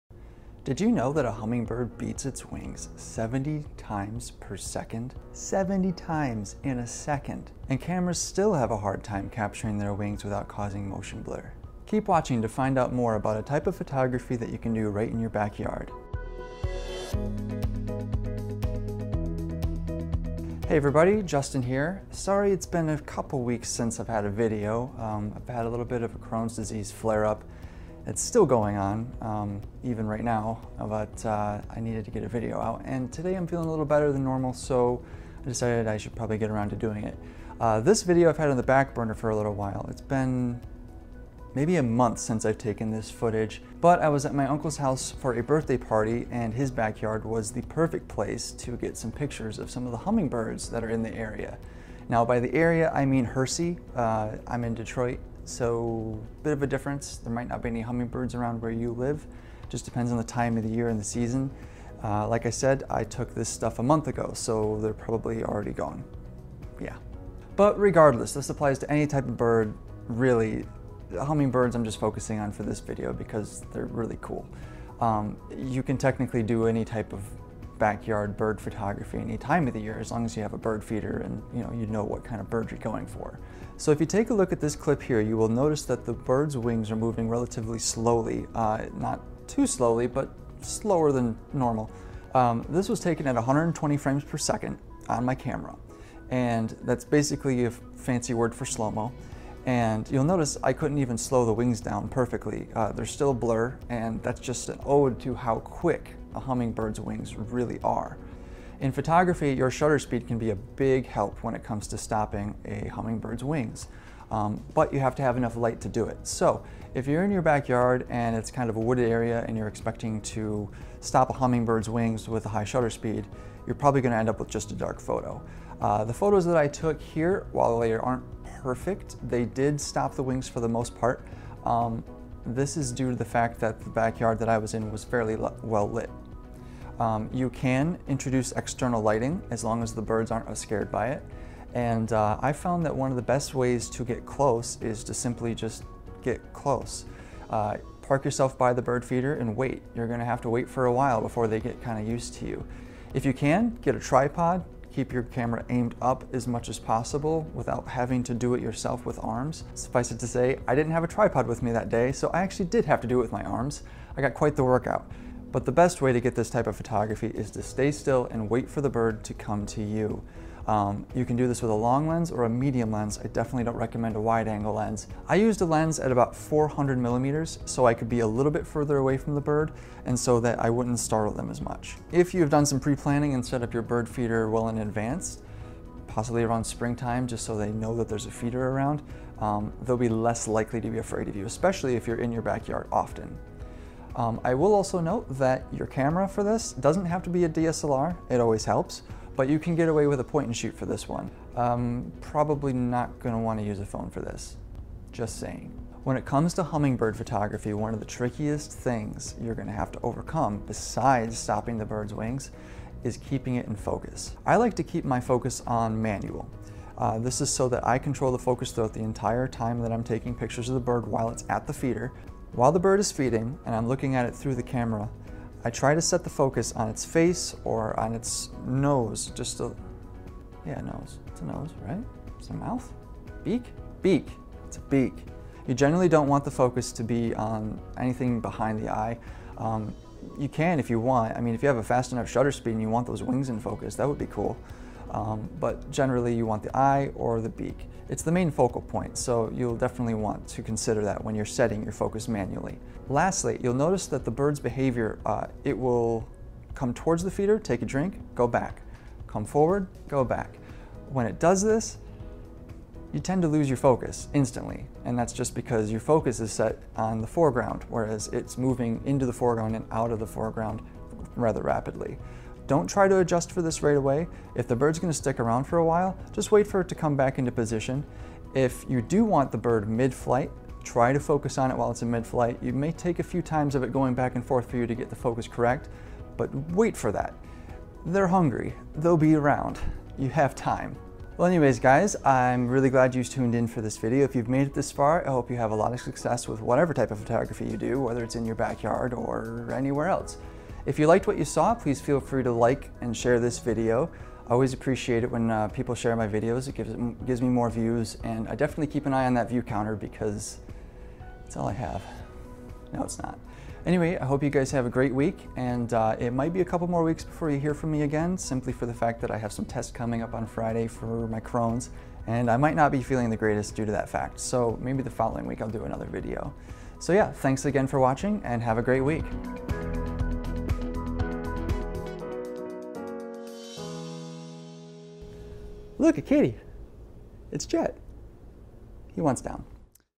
How to take good pictures of hummingbirds
Who doesn't love hummingbirds! Check out this week's video where I take a look at a type of photography you can do right in your own backyard! Also, stick around after the end for a guest appearance!